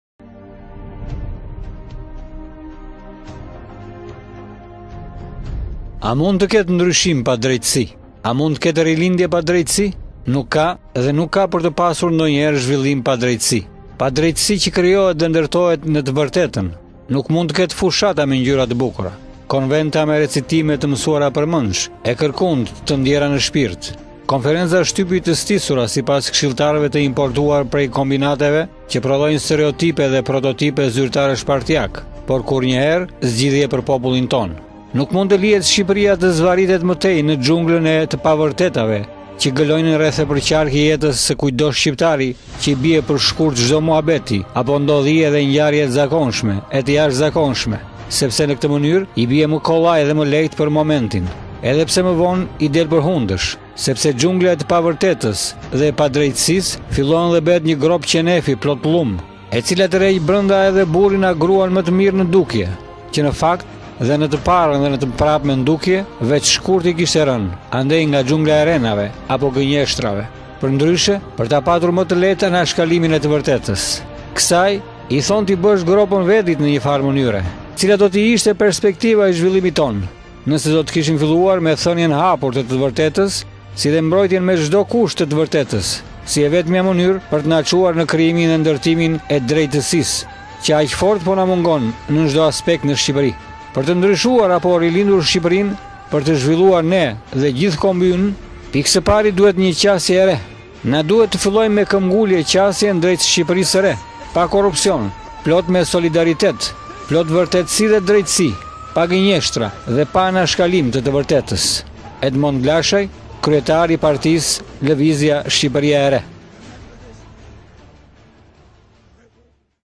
Nuk ka dhe nuk ka per te pasur kurrfare zhvillimi pa Drejtesi!
Pa drejtesine qe krijohet dhe ndertohet ne te verteten, nuk mund te kete fushata me ngjyra te bukura, konventa me recitime te mesuara permendsh e kurrkund te ndjera ne shpirt, konferenca shtypi te stisura sipas keshilltareve te importuar prej kombinateve qe prodhojne stereotipe e prototipe zyrtaresh partiake, por kurrnjehere zgjidhje per popullin tone.
Nuk mund te lihet Shqiperia te zvarritet me tej ne xhunglen e te pavertetave qe gelojne rreth e perqark jetes se kujtdo shqiptari qe i bie per shkurt cdo muhabeti dhe ndodhie a ngjarjeje te zakonshnme a te jashtezakonshme, sepse ne kete menyre i bie me kollaj dhe me lehte per momentin, edhe pse me vone "i del per hundesh", sepse xhungla e te pavertetes dhe e padrejtesise fillon e behet nje grope "qenefi" plot llum, e cila terheq brenda edhe burrin a gruan me te mire ne dukjen e pare, qe ne fakt edhe ne te paren edhe ne te mbrapmen dukje, vec shkurt i kish pas rene, andej nga xhungla e rrenave, apo genjeshtrave perndryshe, per ta pasur me te lehte anashkalimin e te vertetes. Kesaj i thone ti besh gropen vedit, ne nje fare menyre, mirepo do te na duhej qe per tu ndare njehere e mire nga ky llum rrenash, te benim te mundur nje ore e me pare ndarjen e shapit nga sheqeri, apo ndarjen e te vertetes nga genjeshtra apo soji i rrenave.

Per te ndryshuar apo rilindur Shqiperia, per tu zhvilluar ne dhe gjithe kombi yne, pikesepari na duhet nje qasje e Re. Na duhet te fillojme me kembengulje qasjen drejt Shqiperise se Re, pa korrupsion, plot me solidaritet, plot vertetesi dhe drejtesi, pa genjeshtra dhe pa anashkalim te te vertetes.

Edmond Vlashaj, Kryetar i Partise, Levizja Shqiperia e Re.